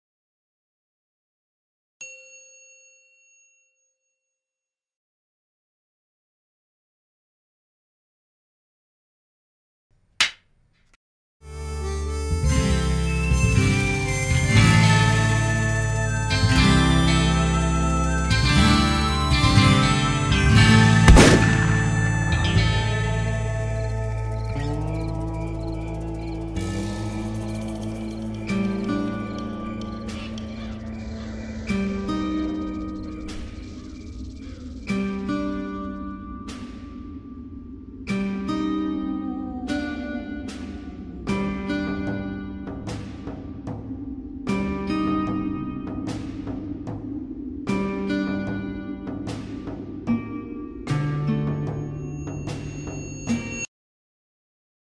Dziki zachód - Grupa Animacyjna CIACH
Pierwsza z tegorocznych animacji stworzonych przez młodzieżową ekipę CIACHu ;)